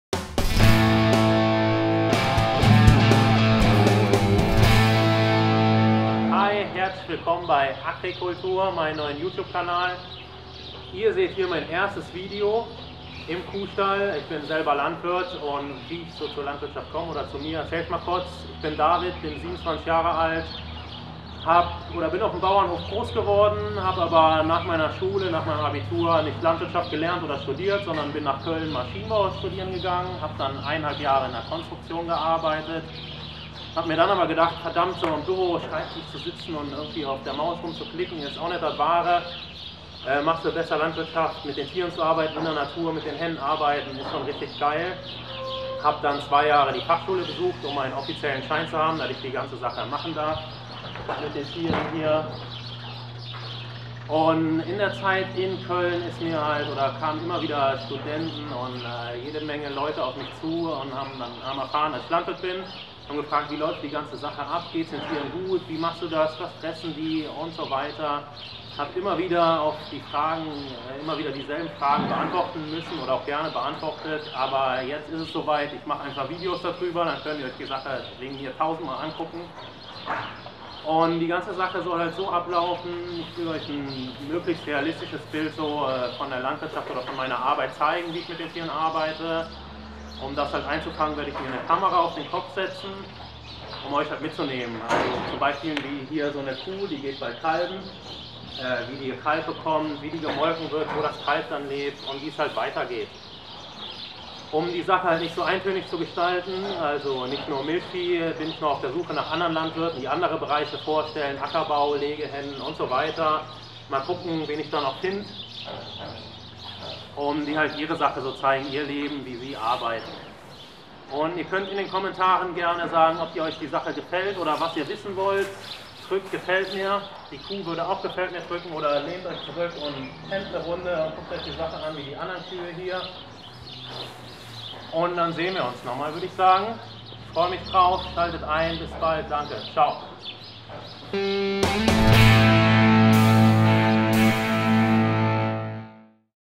agriKULTUR: David ...es geht los! Was ist agriKULTUR und worum geht es?
Hi Leute,
endlich ist es soweit!
Das erste Video von agriKULTUR ist da!
Viele Leute, mit denen ich gesprochen habe, versuchen sich bewusst zu ernähren. Jedoch bestehen gegenüber der Landwirtschaft Vorurteile, die sehr leicht ausgeräumt werden können. Ich möchte dir zeigen, wie ich arbeite und deine Fragen beantworten.
Du interessierst dich dafür, was du täglich isst, weißt aber nicht, woher dein Essen kommt? Bestimmt aus einem Betrieb mit unmöglichen Lebensbedingungen für die Tiere.
Mach dir doch dein eigenes Bild und schalte ein!
Ich freue mich auf eure Fragen. In den nächsten Wochen und Monaten habt ihr die Antworten.